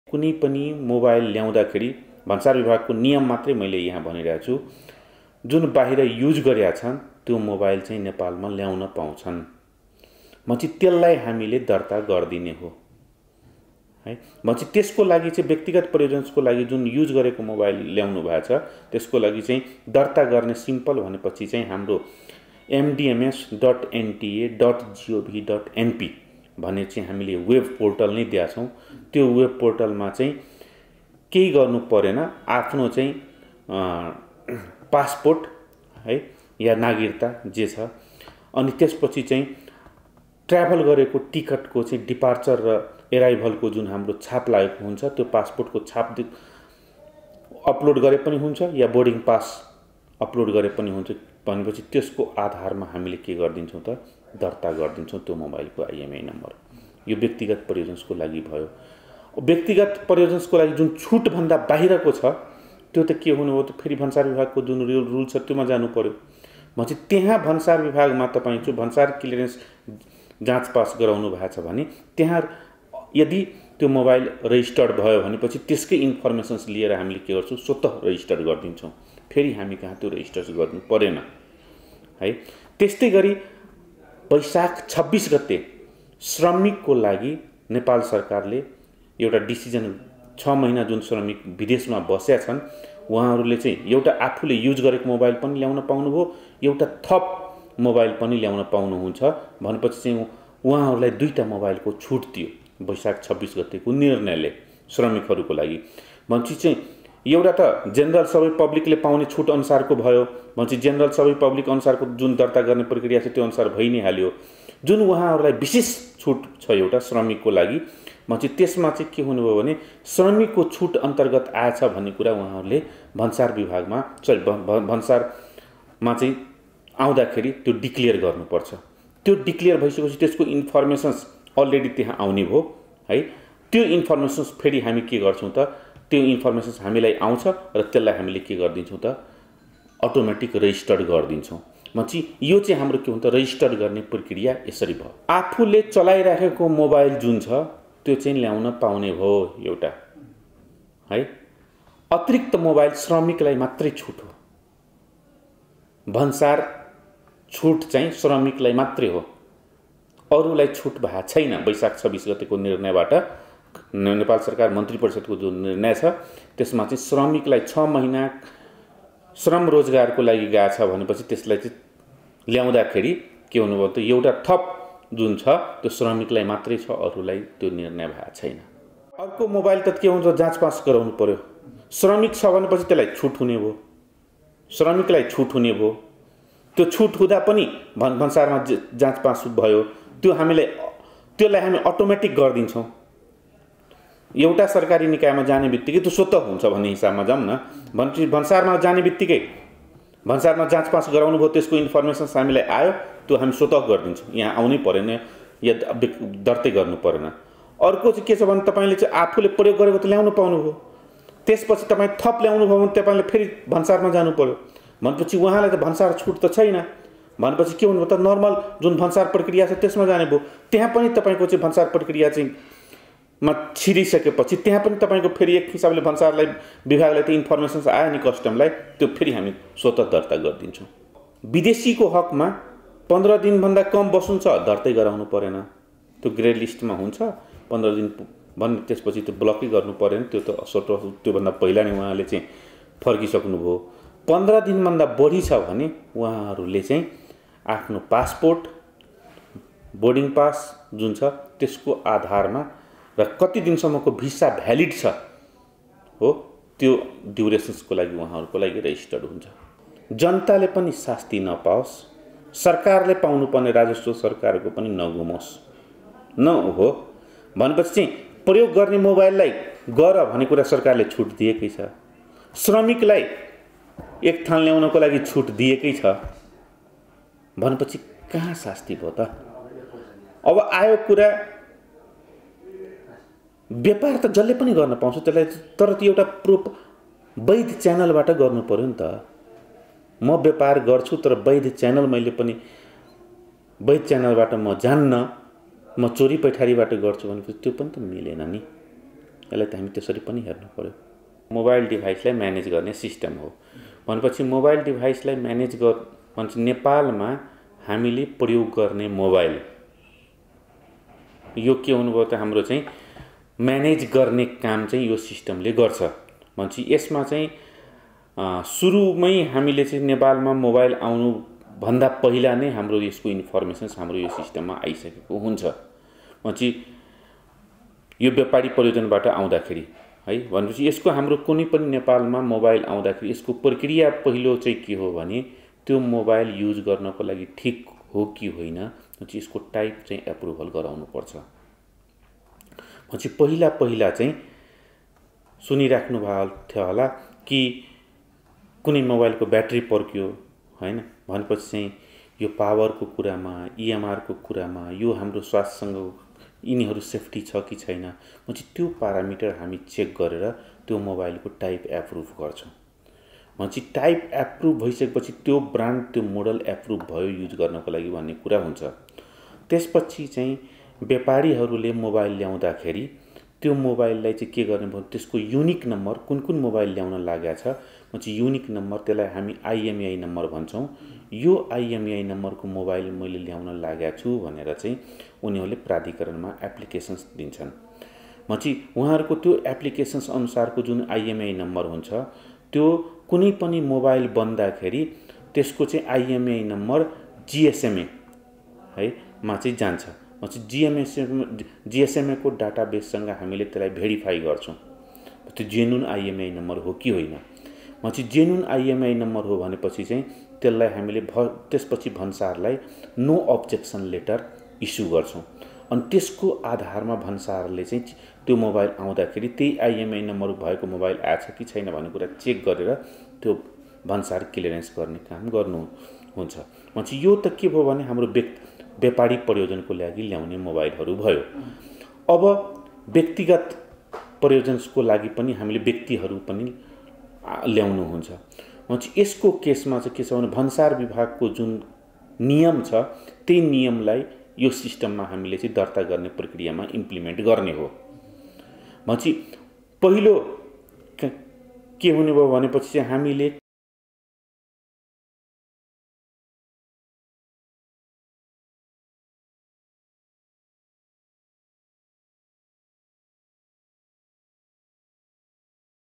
MDMS : मोबाइल दर्ता कसरी गर्ने ? अच्युतानन्द मिश्र, प्रवक्ता, दूरसञ्चार प्राधिकरण
MDMS मा मोबाइल दर्ता कसरी गर्ने ? अच्युतानन्द मिश्र, प्रवक्ता, दूरसञ्चार प्राधिकरण